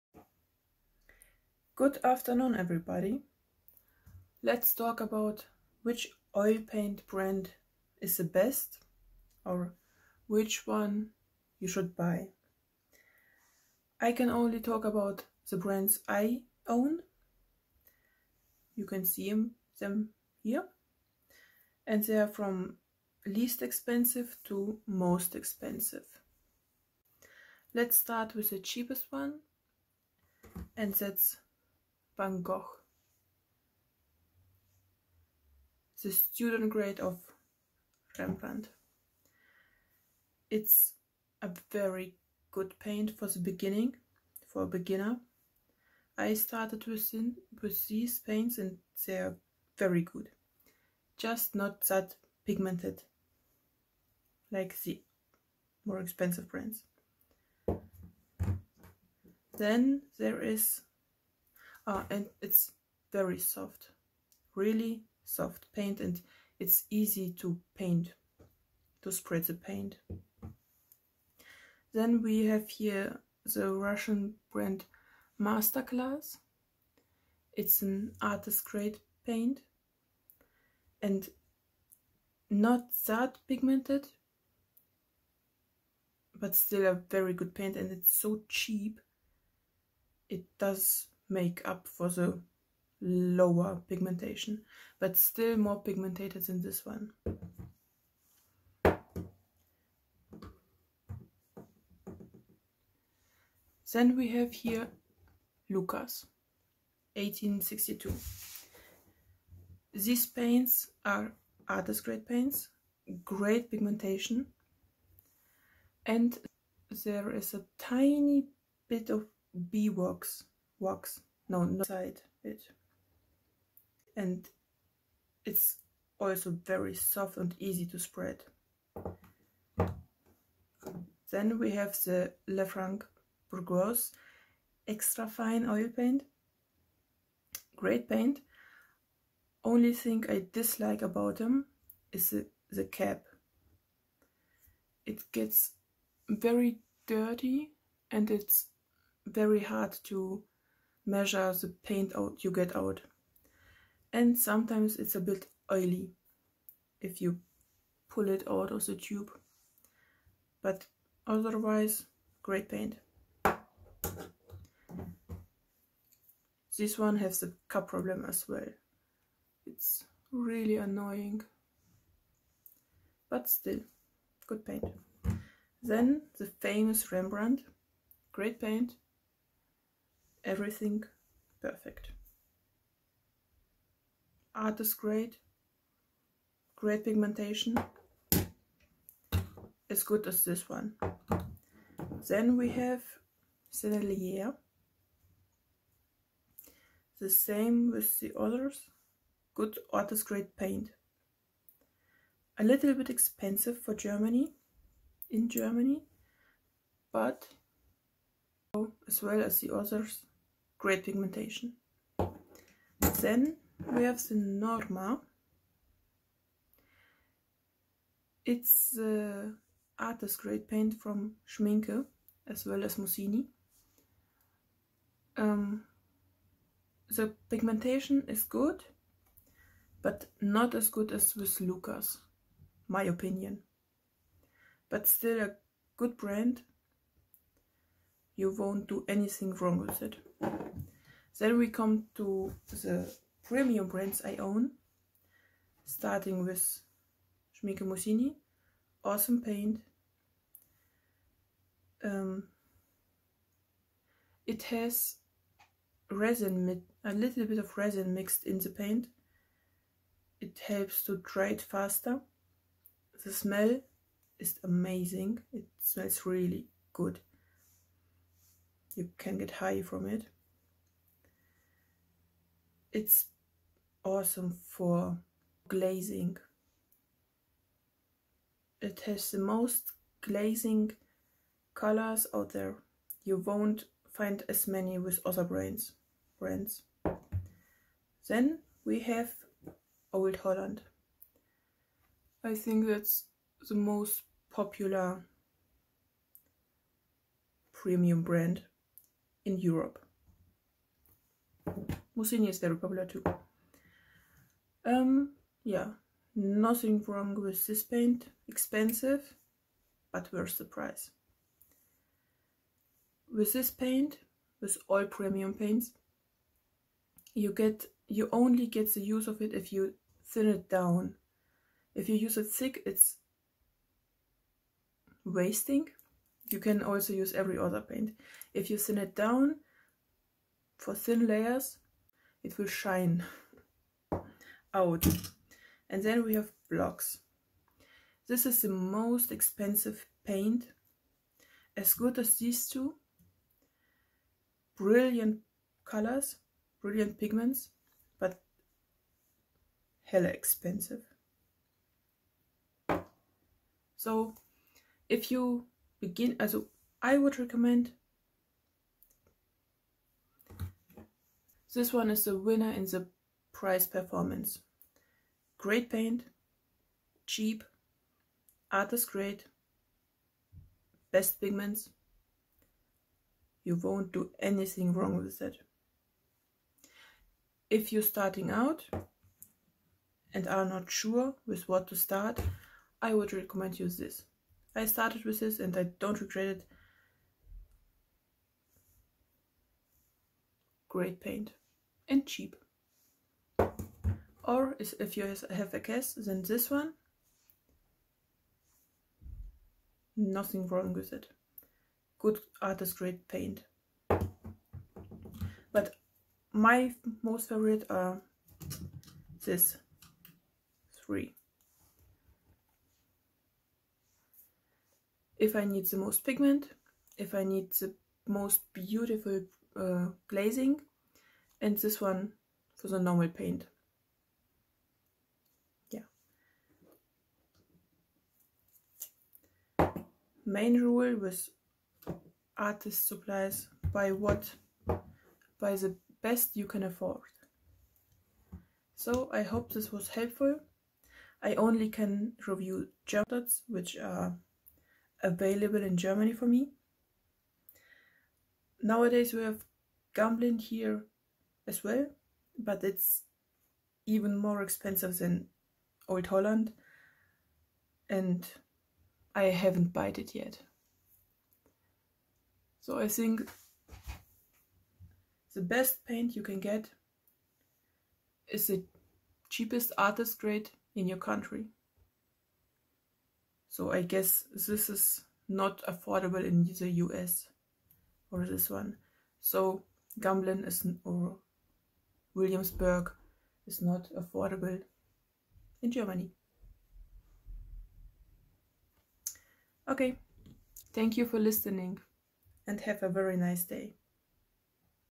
What Oil Paint Brand to buy/ Oil Paint Brand Comparison
There are many Oil painting Brands out there, in this Video I want to show you a small sample available in Germany. My favourites and what I dont like about them.

Oil Paint Brands shown in the Video

Van Gogh Oil Paint
Masterclass Oil Paint
Lukas 1862
Lefranc & Bourgeois extra fine
Sennelier
Schmincke Norma
Schmincke Mussini
Old Holland
Blockx